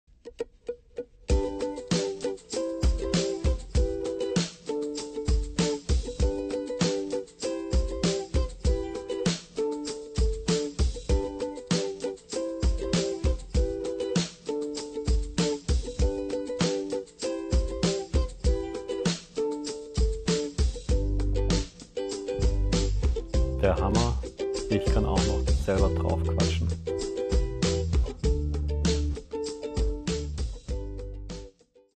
Adobe Spark Beispiel
Inhalt mehr oder weniger sinnvoll, es geht ums Prinzip! Mit Adobe Spark ist so ein Video in wenigen Minuten fertig.